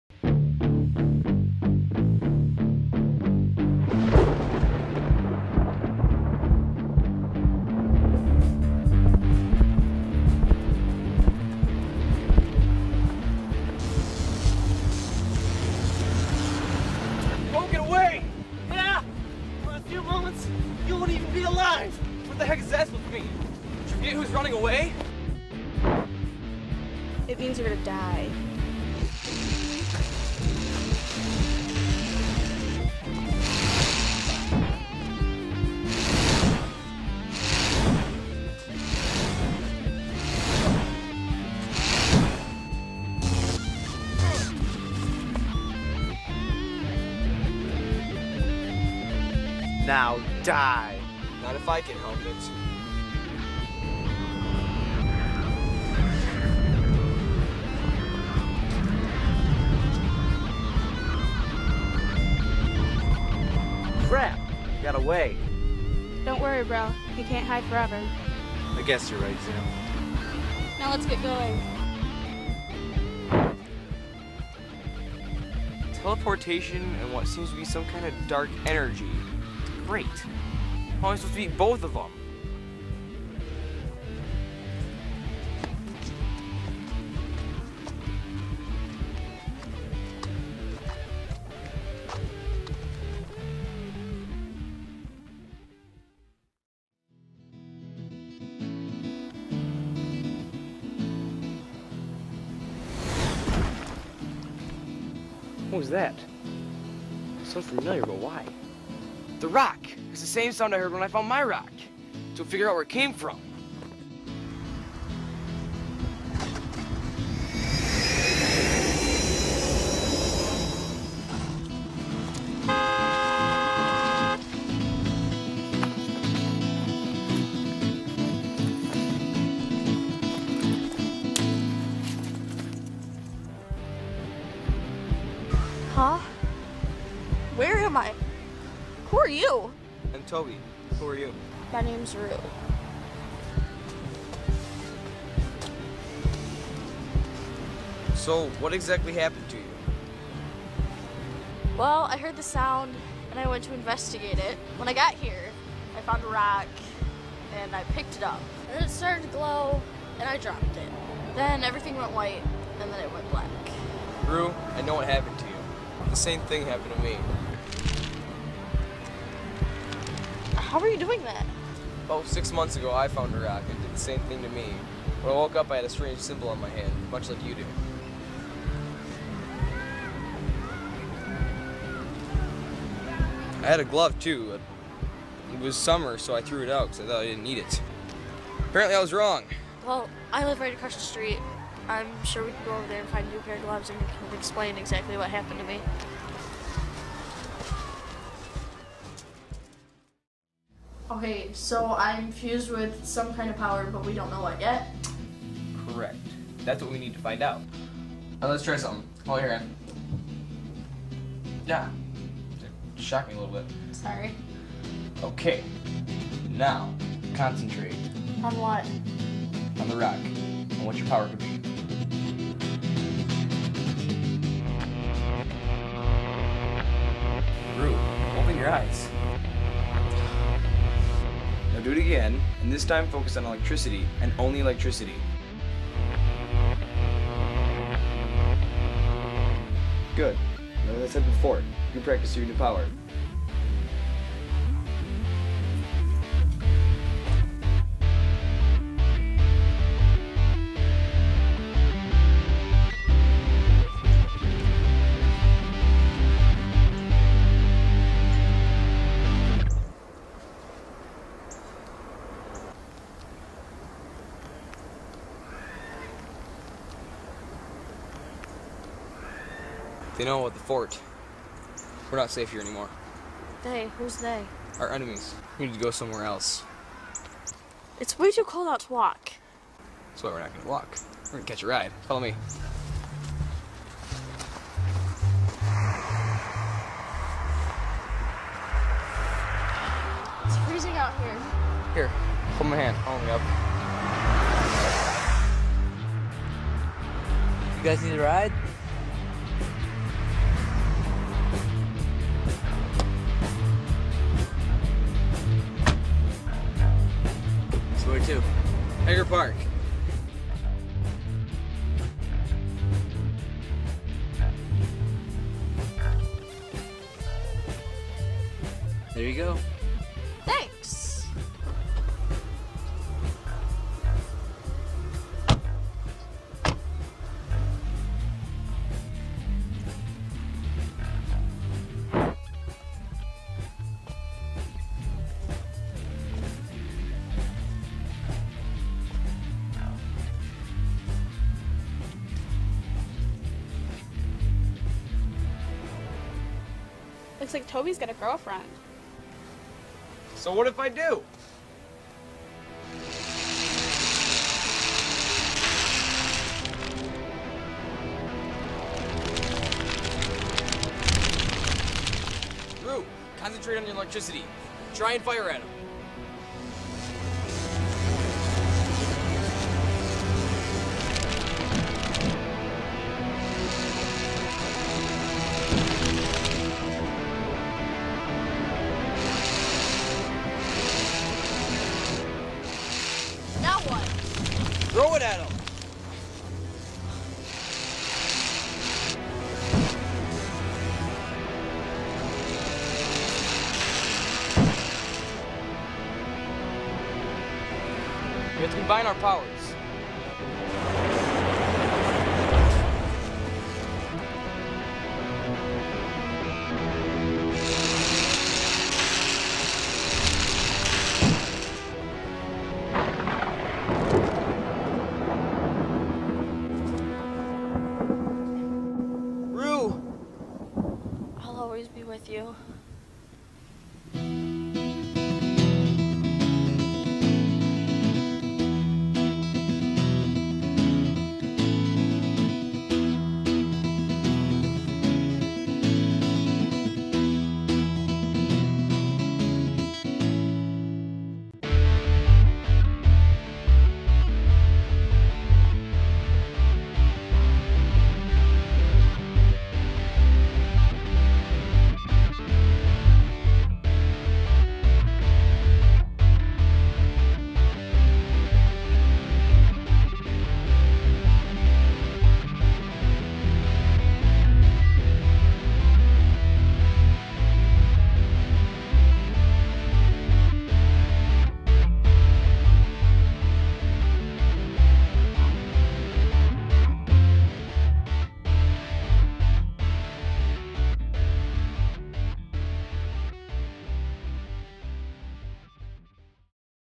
Tayza 2: Veil of Darkness
This was my second short film. It was created for a local film festival in 2004 and took first place.

Tayza is about a young teen, fused with elemental powers, who must defeat the evil intent on taking over the world.